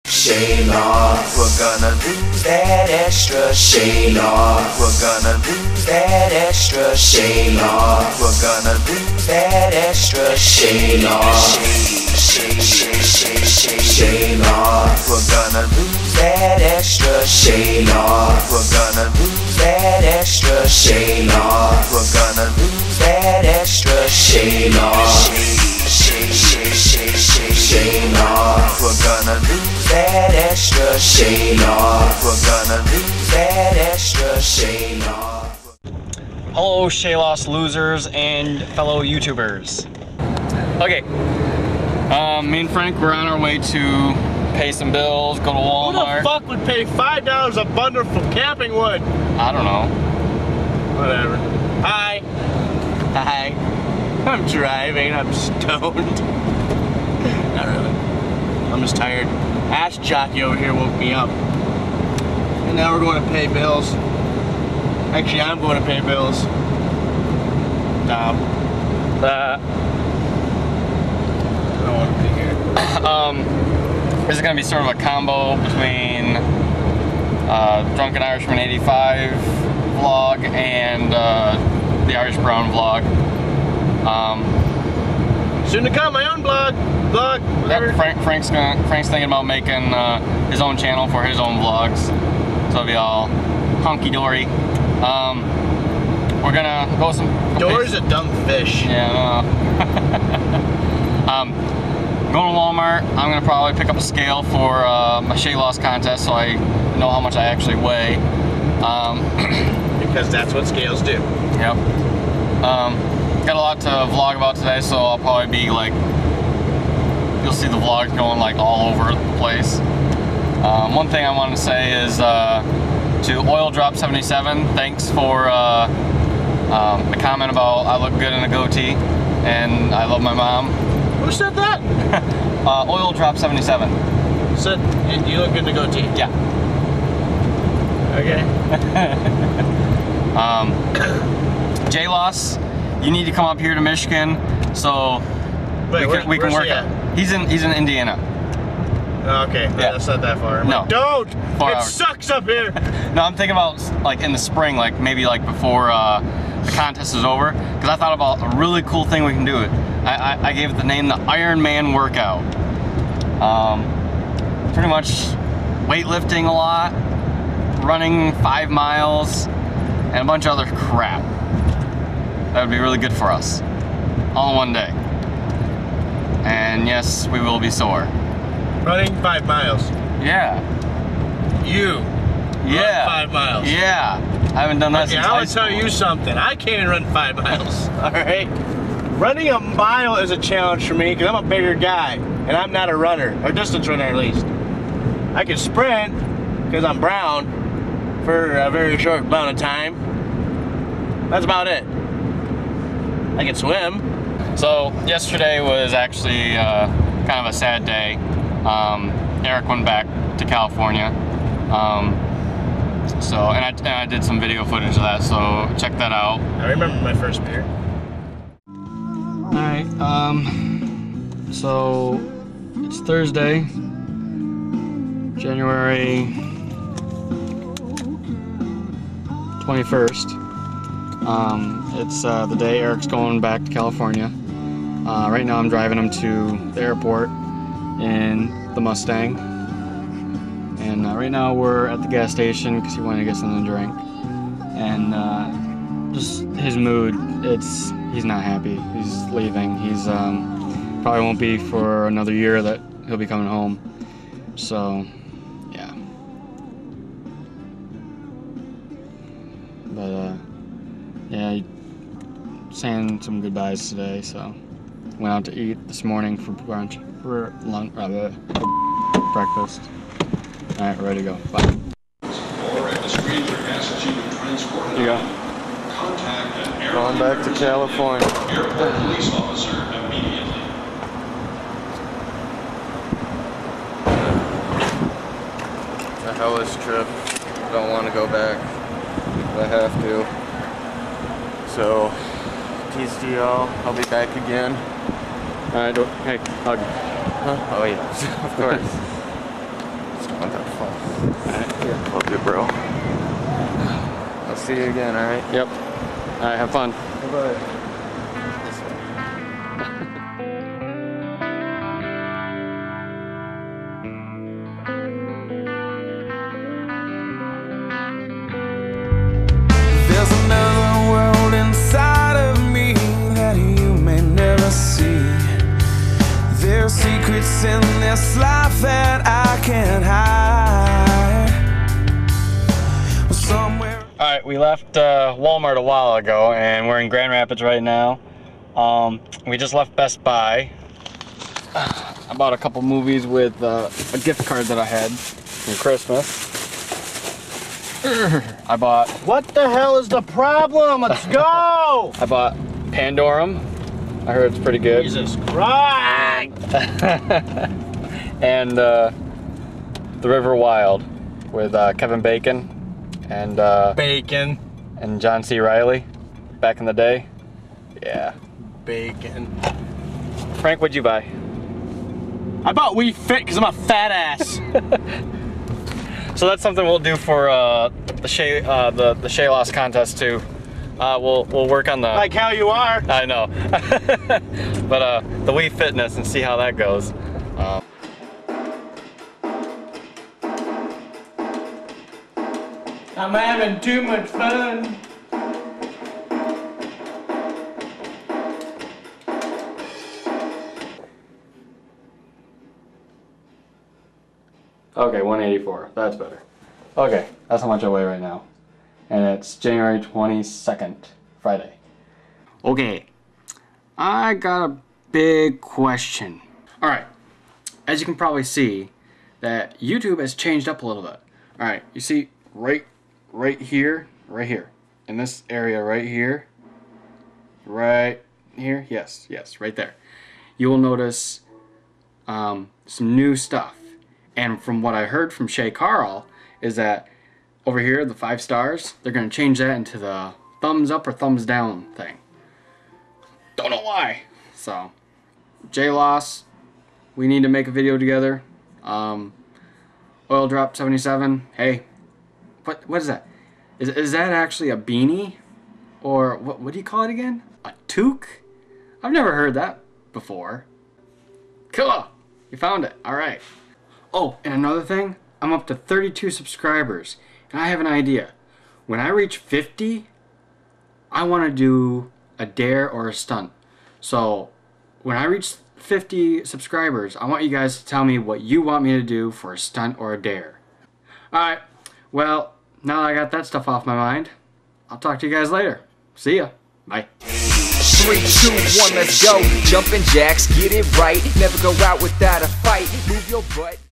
Shayloss Vlog #6
Vlog 6 is a bunch of things that i needed to talk about, from Eric going back to California, to what I'm gonna do when i reach 50 subs.